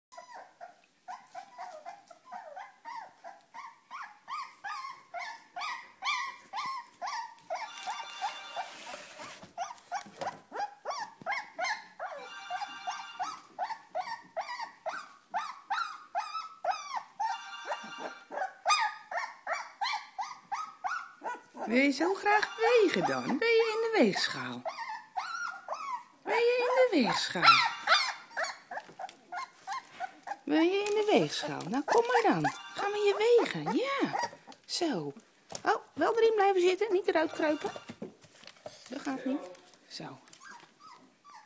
Pup weet wat ie wil.
Pup wil in de weegschaal.